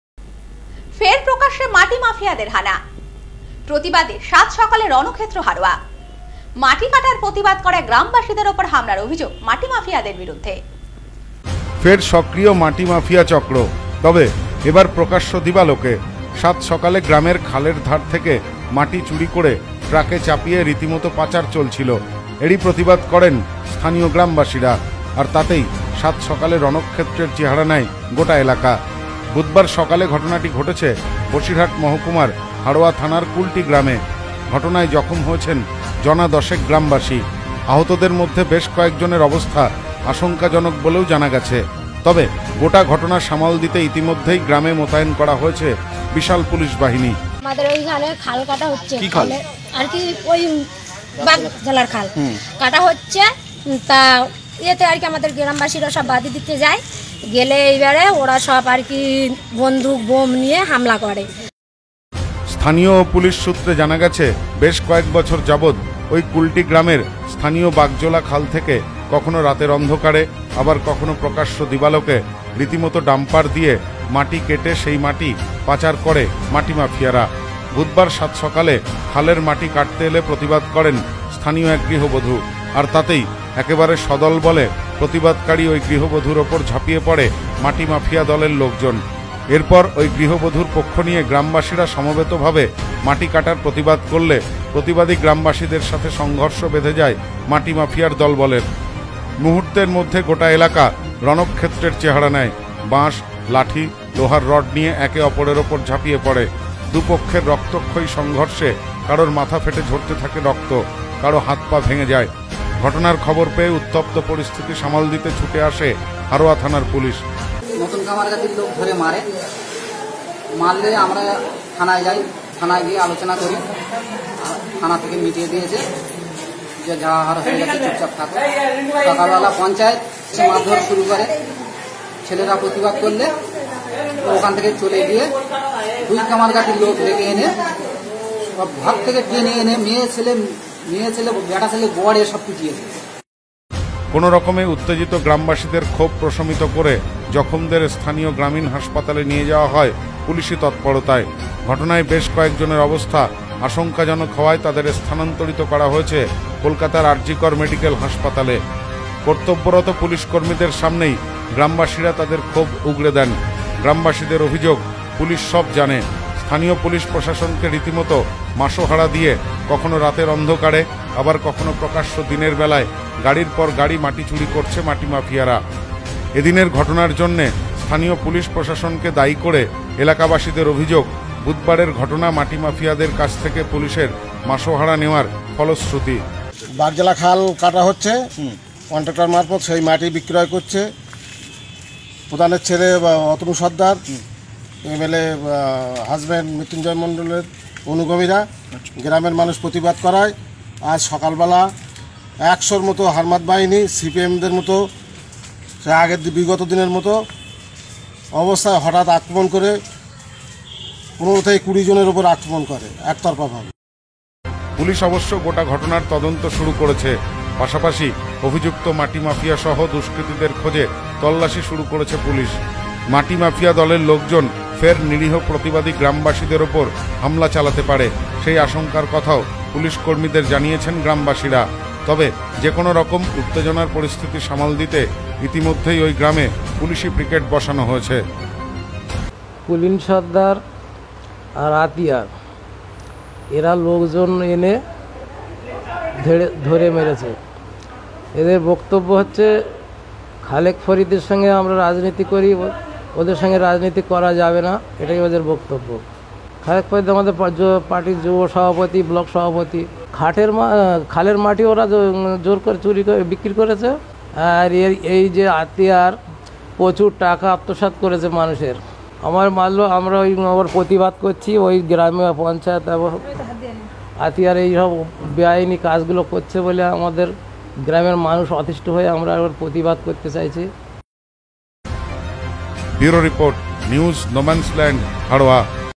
ফের প্রকাশ্যে মাটি মাফিয়ার হানা৷
ফের প্রকাশ্যে মাটি মাফিয়ার হানা, প্রতিবাদে সাত সকালে রণক্ষেত্র হাড়োয়া ৷ মাটি কাটার প্রতিবাদ করায় গ্রামবাসীদের উপর হামলার অভিযোগ৷